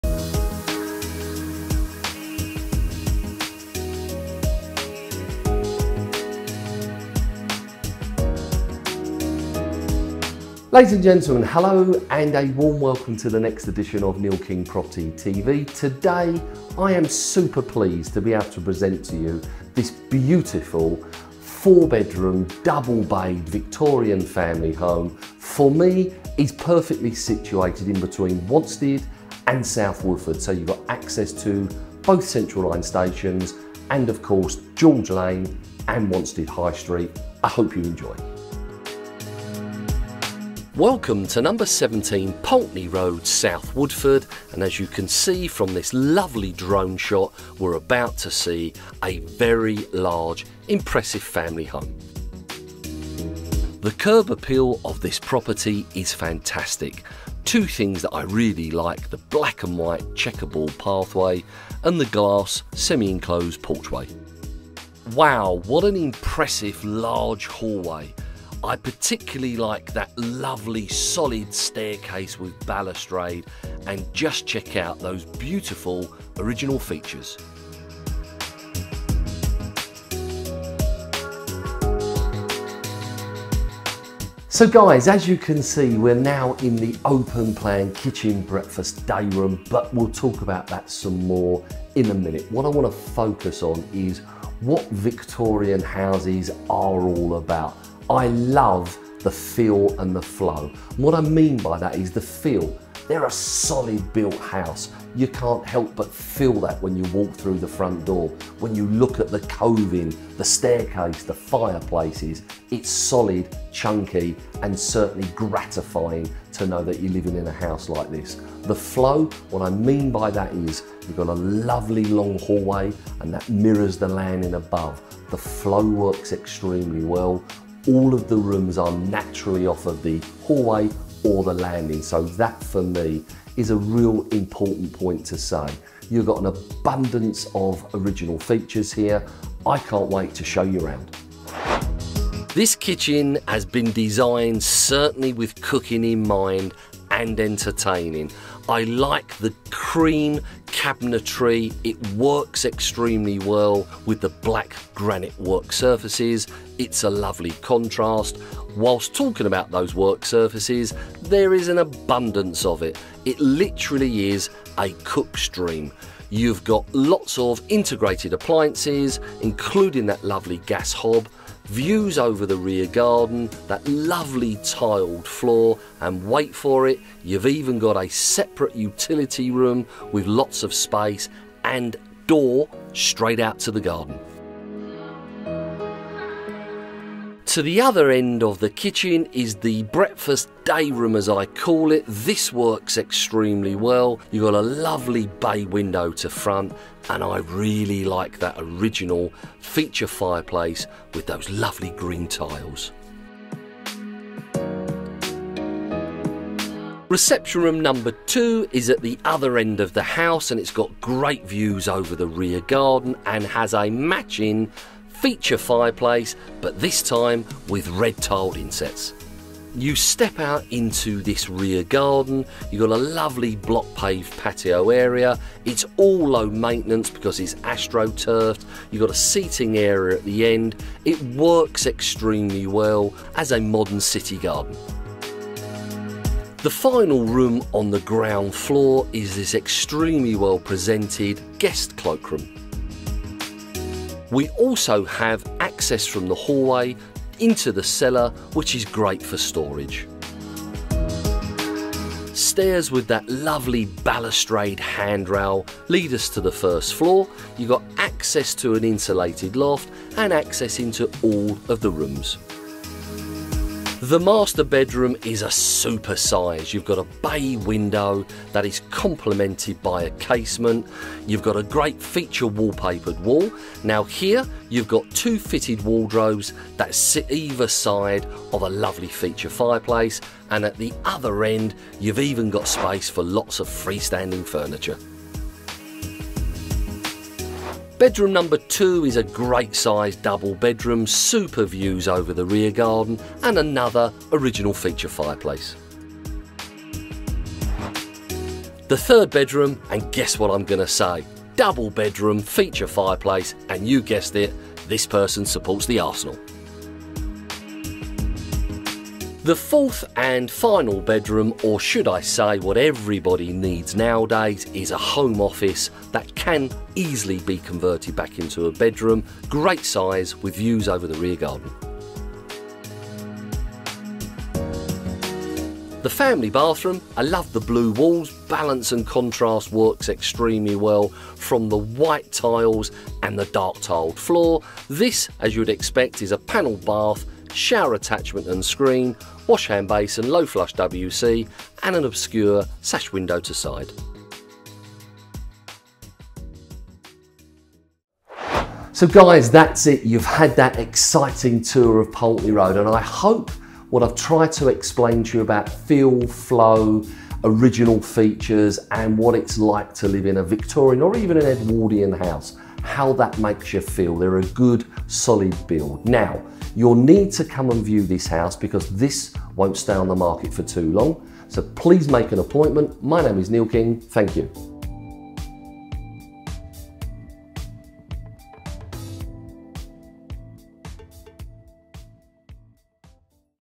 Pulteney Rd, South Woodford, London E18
Pulteney Road is situated in a perfect location close to both South Woodford and Wanstead Central Line stations with direct links into the City and West End, and easy access to a host of high street shops, coffee bars, restaurants and Christ Church Green.

This attractive well proportioned four bedroom period family home is decorated and maintained to a high standard and offers superb living accommodation arranged over two floors. The ground floor offers a well balanced kitchen/diner/day room measuring almost 24ft and makes the ideal entertaining hub for any family, complemented by a utility room and guest cloakroom.  The lounge offers great views over the low maintenance rear garden and the final area accessed from the ground floor is the cellar. The first floor boasts four well proportioned bedrooms with the huge  master measuring just over 17ft with a range of fitted wardrobes, an internal viewing is highly recommended to appreciate the many additional fine features.